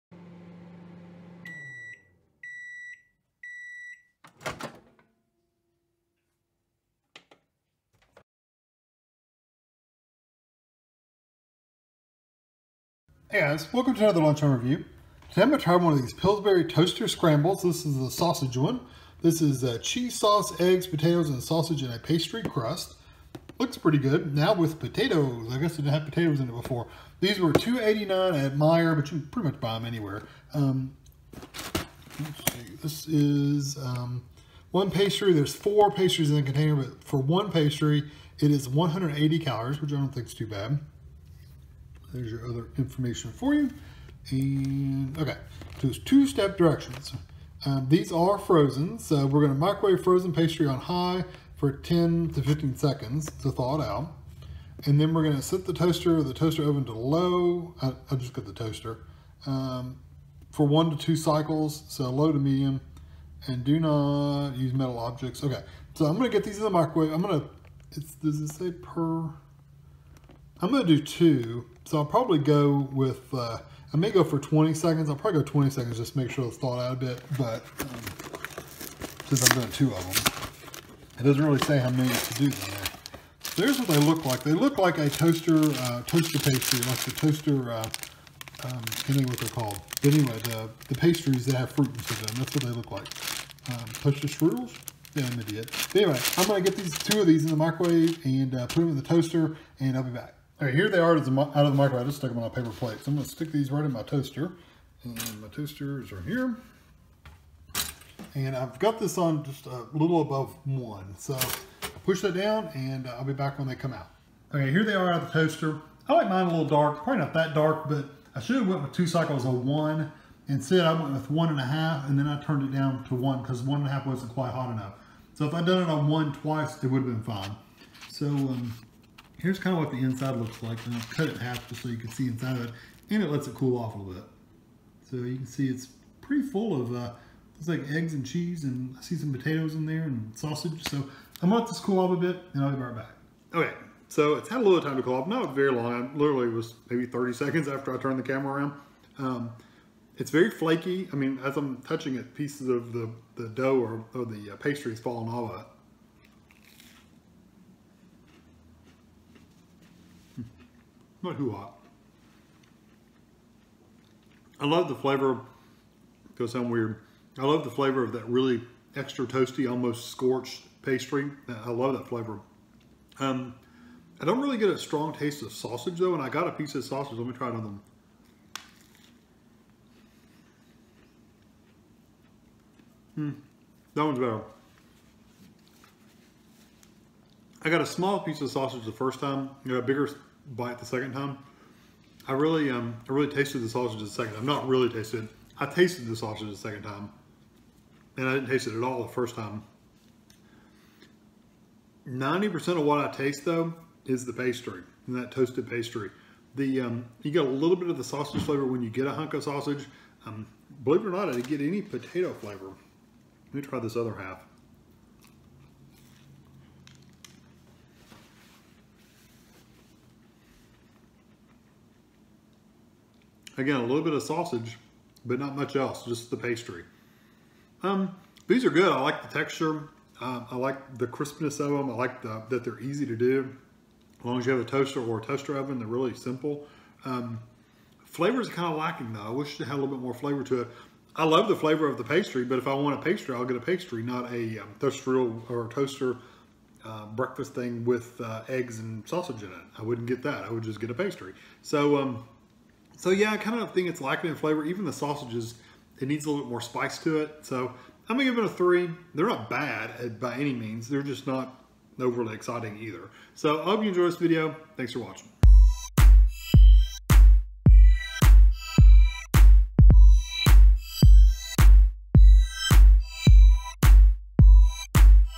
Pillsbury Toaster Scrambles: Sausage Review
This is a taste test/review of the Sausage Pillsbury Toaster Scrambles. They are cheese sauce, egg, potatoes and sausage in a pastry crust. They were $2.89 at Meijer. 1 pastry = 180 calories